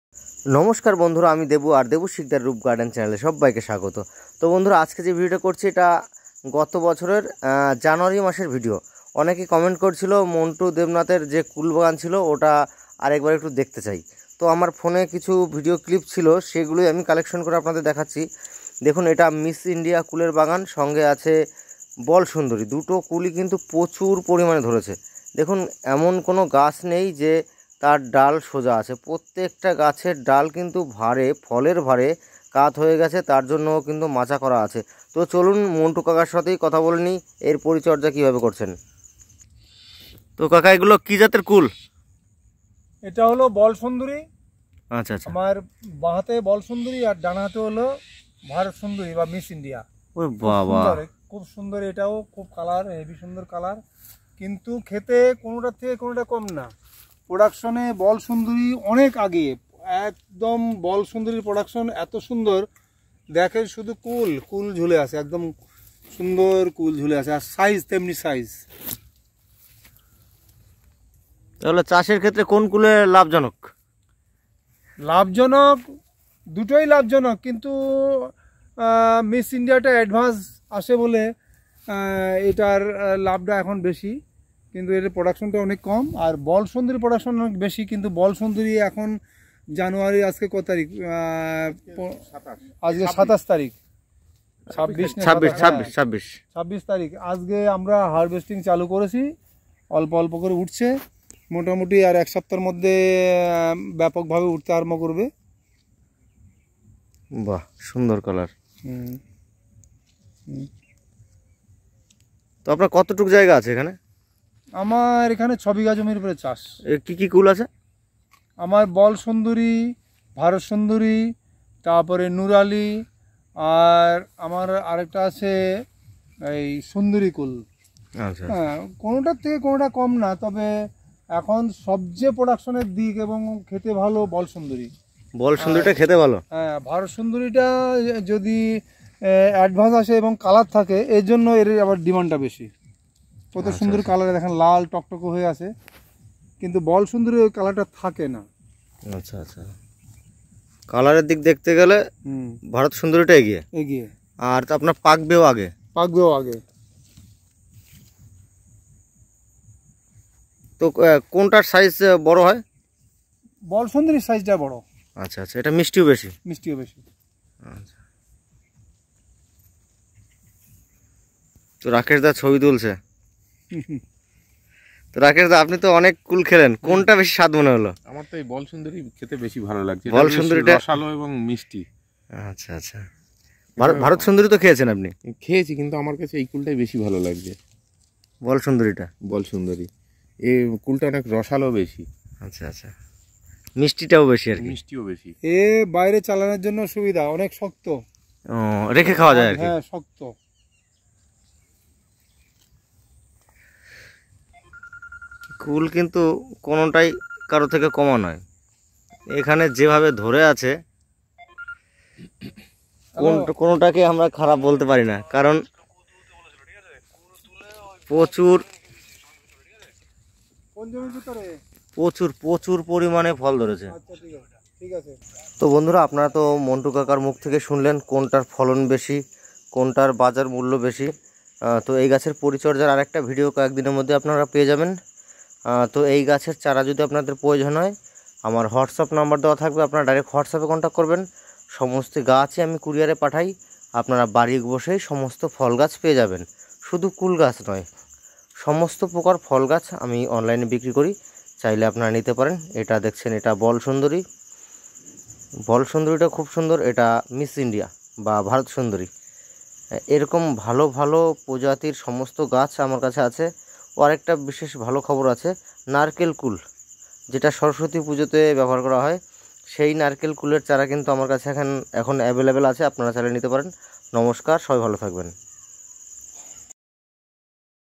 মিস ইন্ডিয়া/ভারত সুন্দরী নাকি বলসুন্দরী কোন টা চাষের জন্য সঠিক জেনে নিন
Debu sikdar.8250964154##
যে কোনো দেশী ও বিদেশি ফলের গাছ অনলাইনে। বাড়ি বসে পেতে
 আজই যোগাযোগ করুন দেবু সিকদার
বাদকুল্লা, নদীয়া